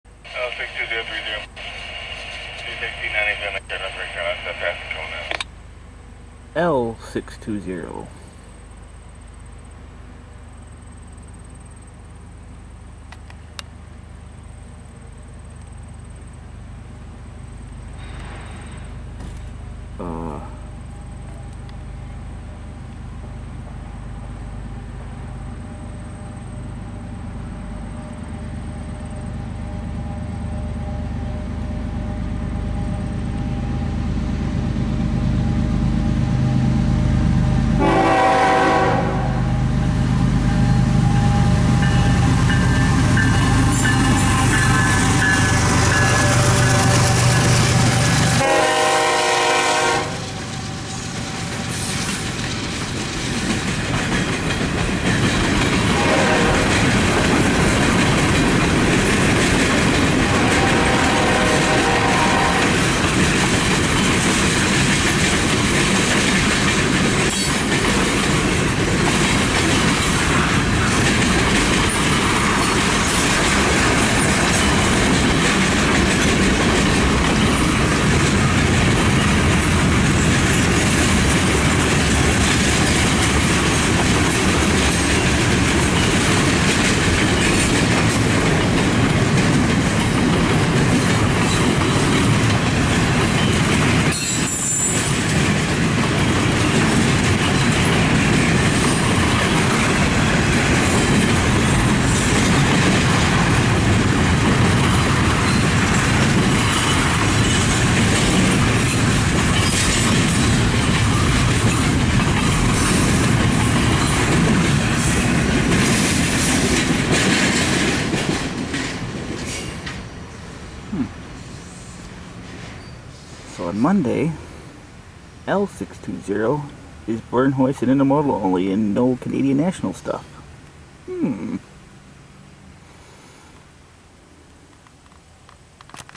CSX L620 in Lacona NY...7-30-2018
Train is north bound with 23 cars. 2:23pm